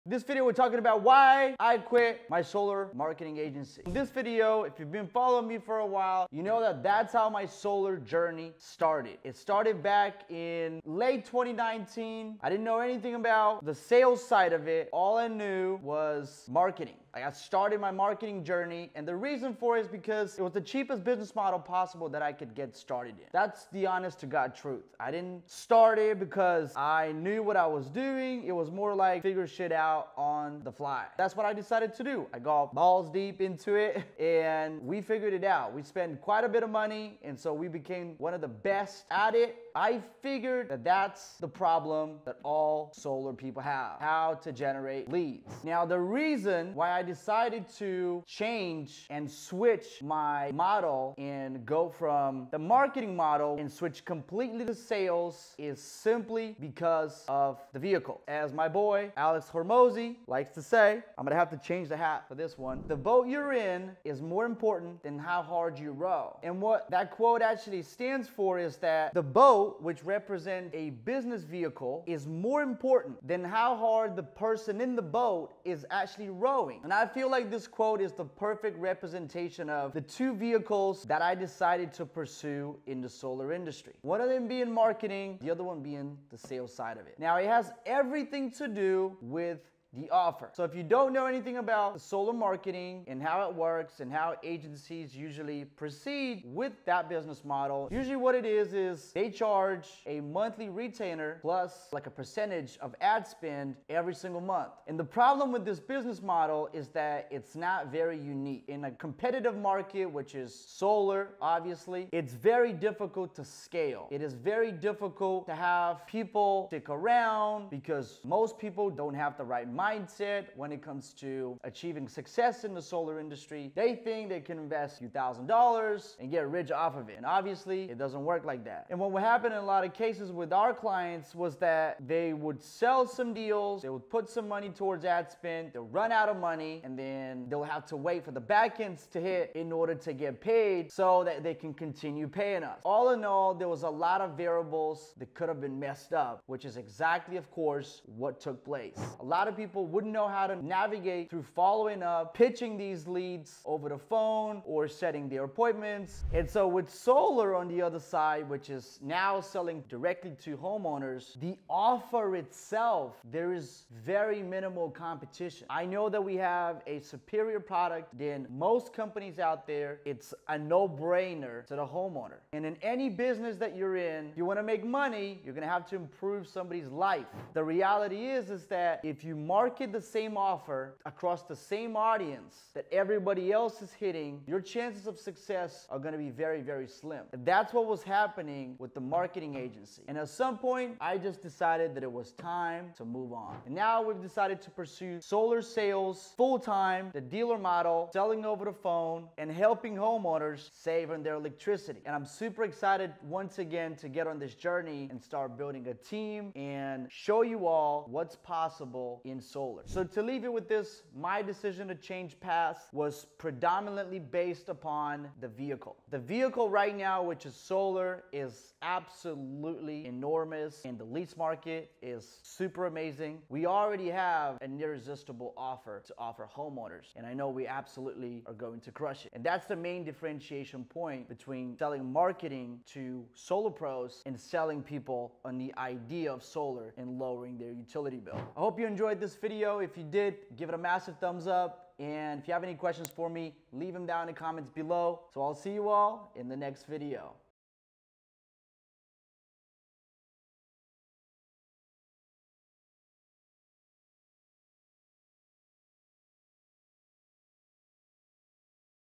Why I Quit My Solar Marketing Agency
In this video, I'm telling you why I quit my solar marketing agency.

Brand New to Solar?